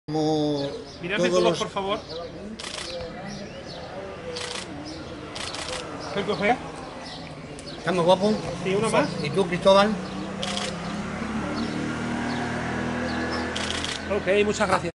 Recepcion de los talones
El Presidente de Cáritas y el responsable del Centro de Acogida de Badajoz recogen los talones y expresan su agradecimiento a los participantes y donantes este año.